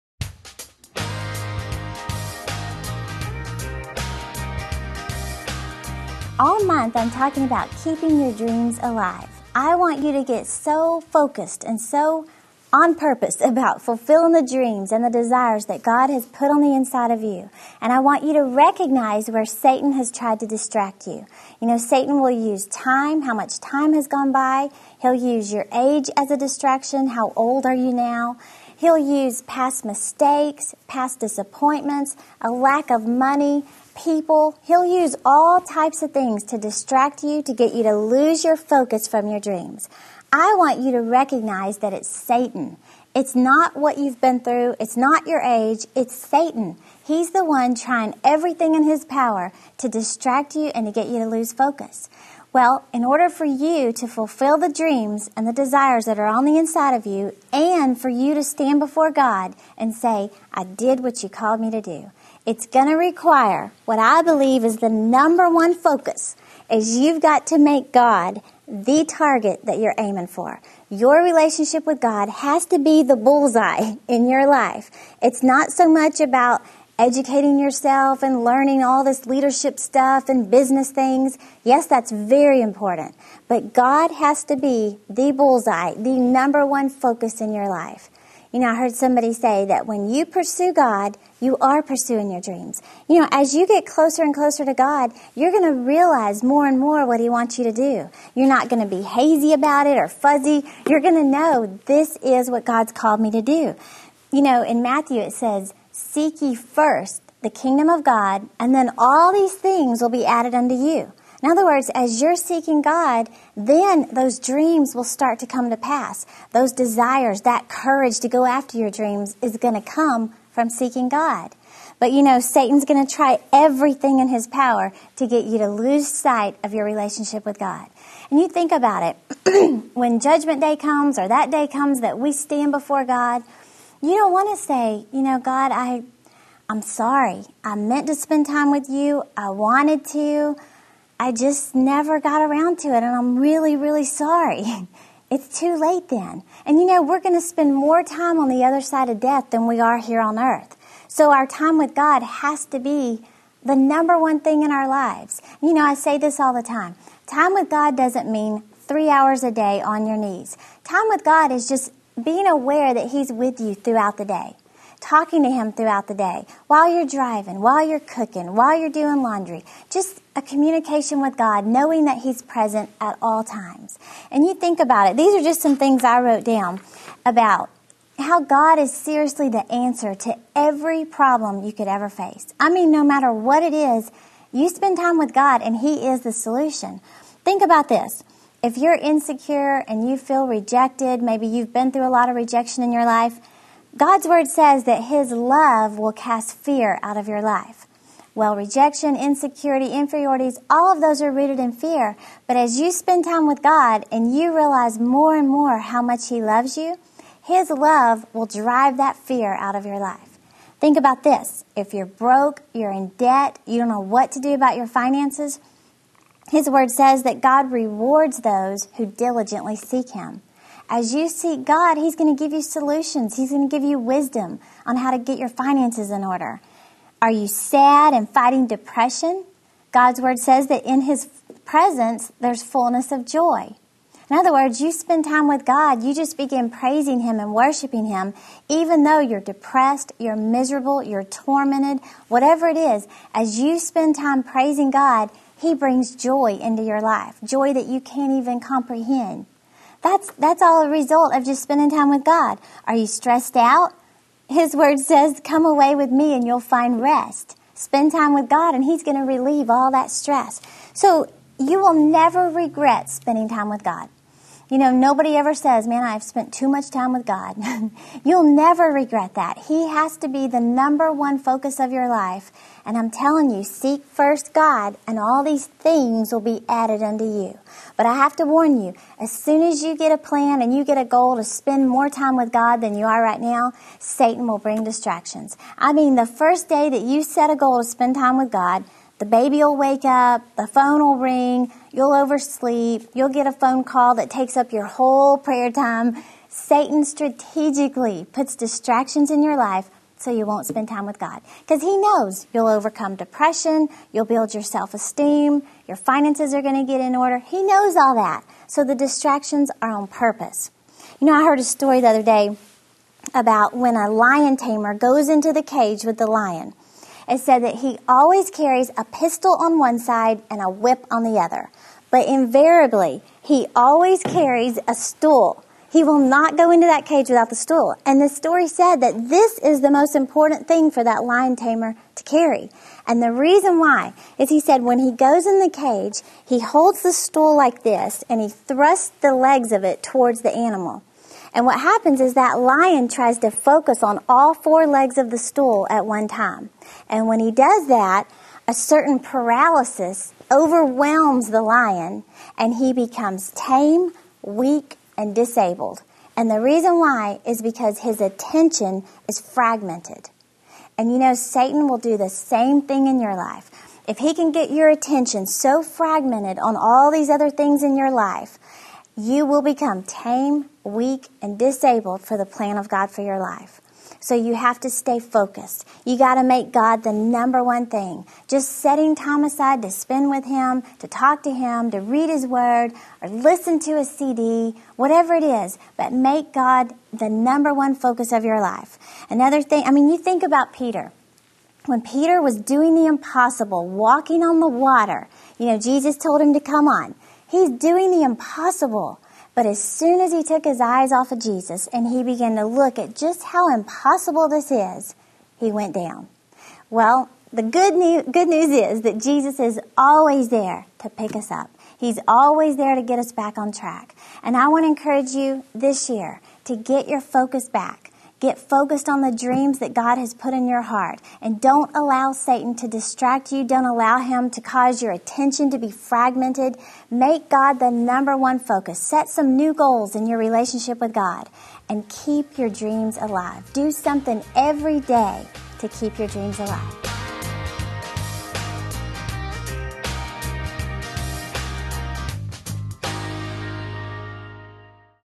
Keep Your Dreams Alive, pt.3
Recognize the distractions that could keep you from pursuing and achieving your dreams.